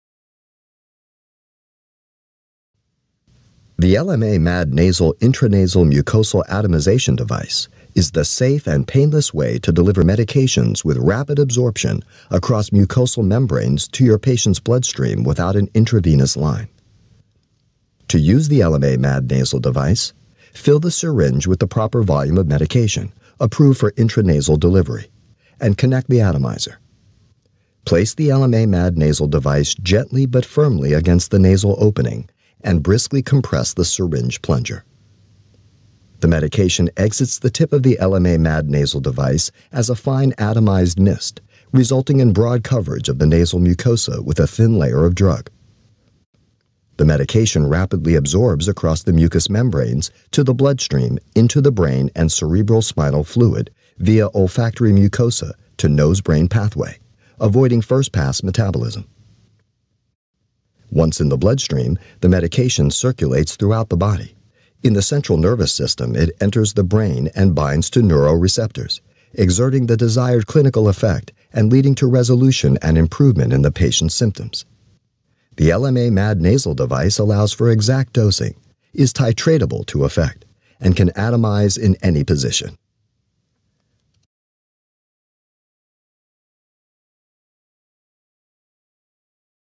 How Teleflex MAD Nasal Intranasal Mucosal Atomization Device Works?
Teleflex MAD Nasal Intranasal Mucosal Atomization Device provides a safe and painless method for delivering medications. It can atomize in any position, and its soft, conical plug on the tip creates a seal with the nostril, preventing fluid from being expelled. The spray transforms drugs into a fine mist of particles ranging from 30 to 100 microns in size.

Teleflex MAD Nasal Intranasal Mucosal Atomization Device-